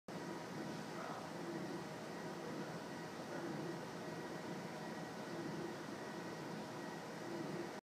GovDeals: Spirit CT800 Treadmill
Spirit CT800 Treadmill sold by University of Pittsburgh, PA on GovDeals.com